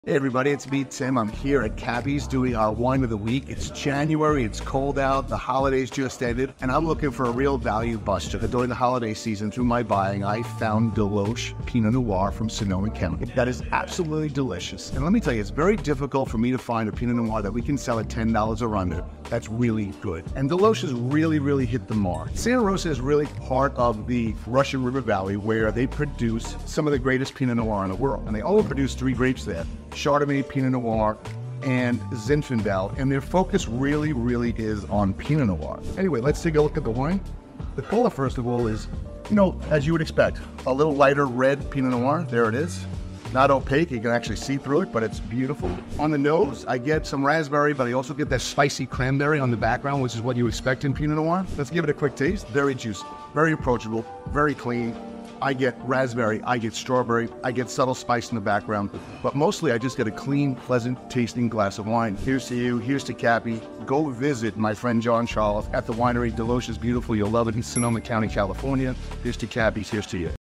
🍷 Wine of the Week: DeLoach Pinot Noir! 🌟
January calls for warmth, and we've found the cozy answer! ❄️🔥

💰 A Steal at $9.97 (Reg. price of 12.99), Unbelievably Good — Value Buster Alert!
🍇 Sonoma County's Gem — The Heartbeat of Russian River Valley
🍒 Raspberry Elegance, Spicy Cranberry Symphony in Every Sip
🌈 Approachable, Juicy, and Clean — Pinot Noir Bliss!

Visit Cappy's, Dive into the Charm of Deloach Pinot Noir! 🍷🎉

🚗 Local delivery & nationwide shipping available!
😎 Spirits high, prices low. That's how we roll!